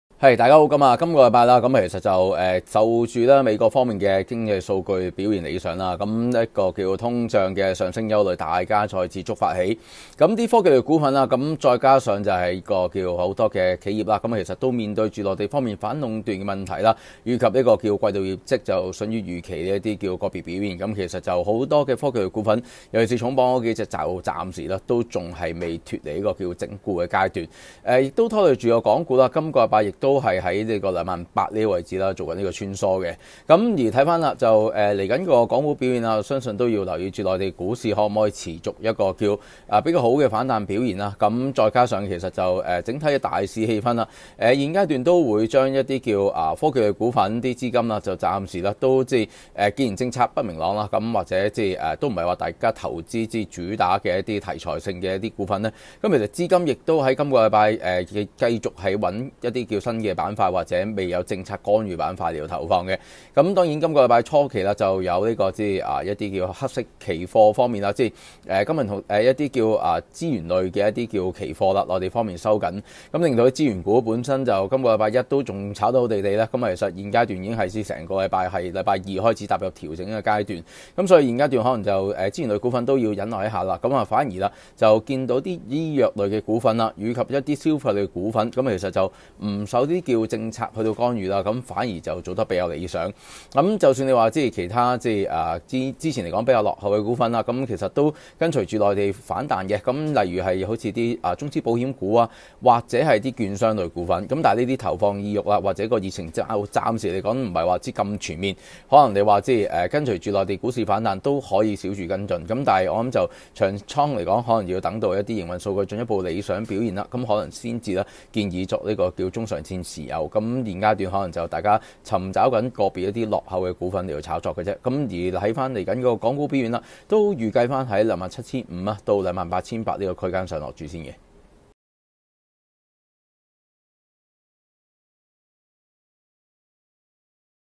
李偉傑專家短評｜科技股未脫離整固 資金流向哪些板塊？
科技類股份未脫離整固階段，資金繼續流向新板塊，特別是不受政策干預的板塊，當中以哪些類別股份的表現比較理想？